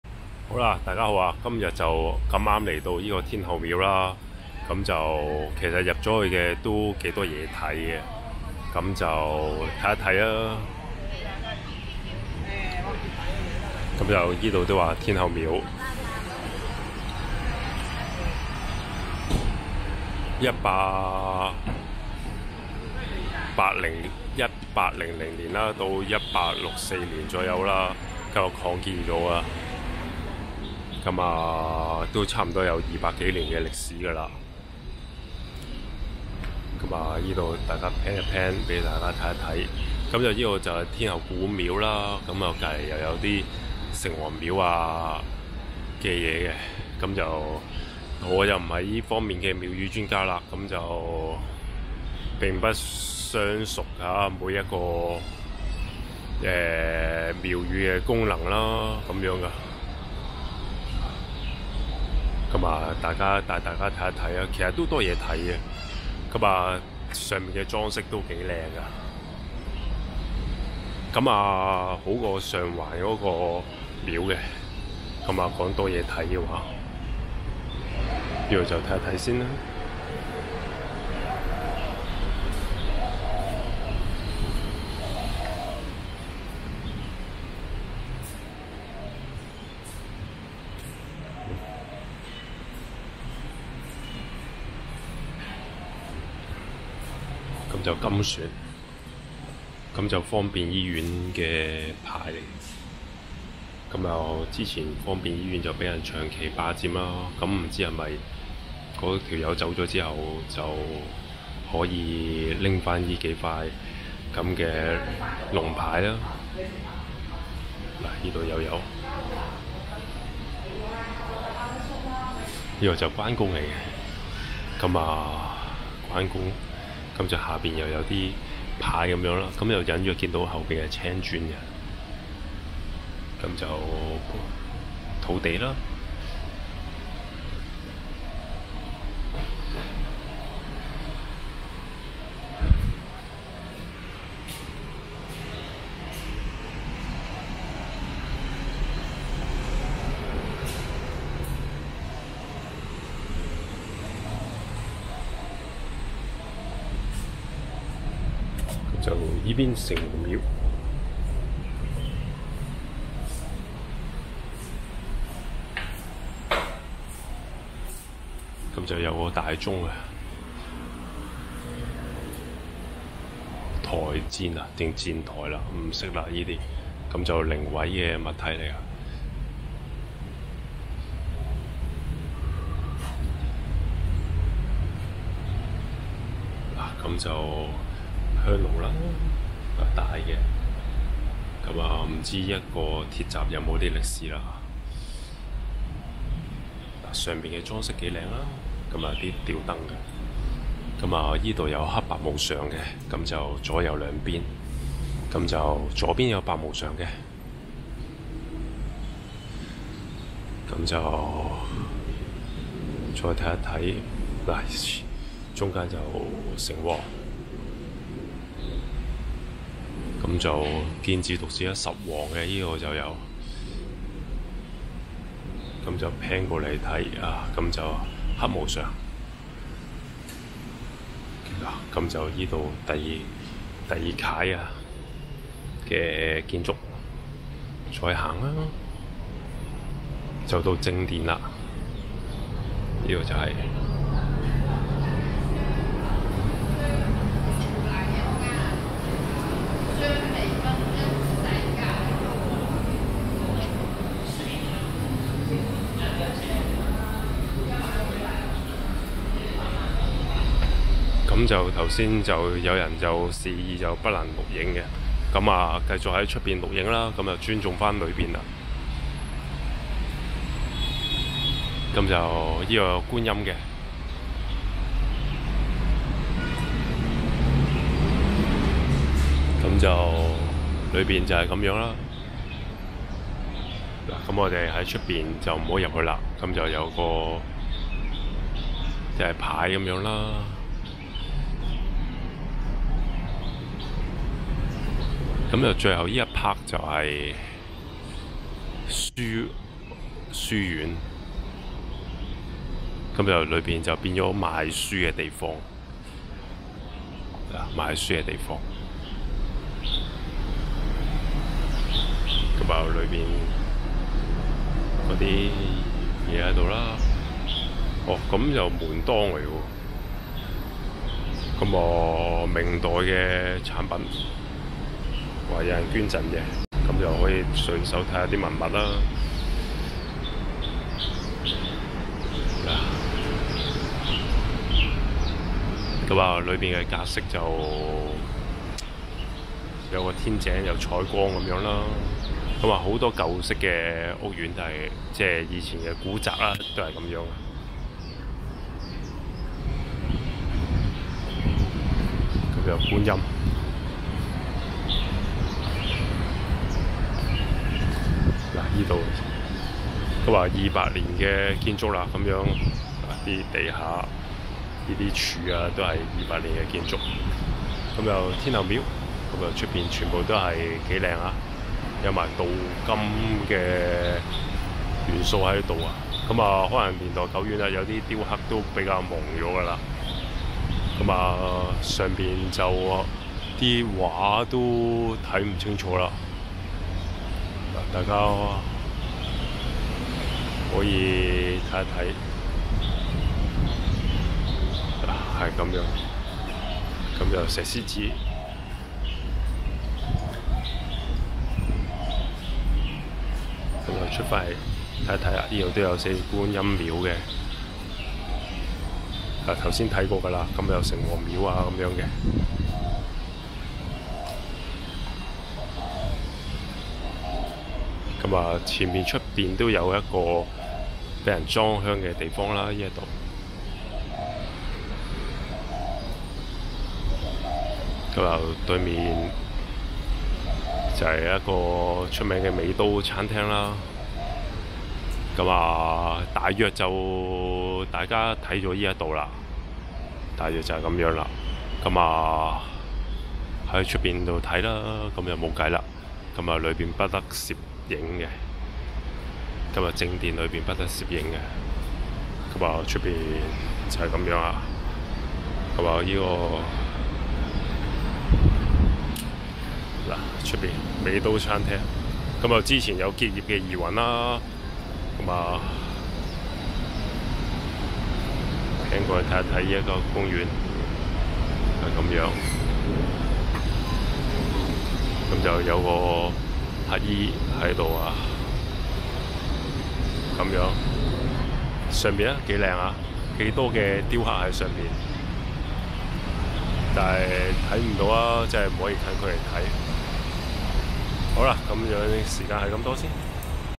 「香港旅遊」油麻地天后廟
油麻地天后廟是香港九龍規模最大的天后廟，位於油麻地廟街近眾坊街一帶，現由東華三院委託華人廟宇委員會管理，現被古物古蹟辦事處列為一級歷史建築。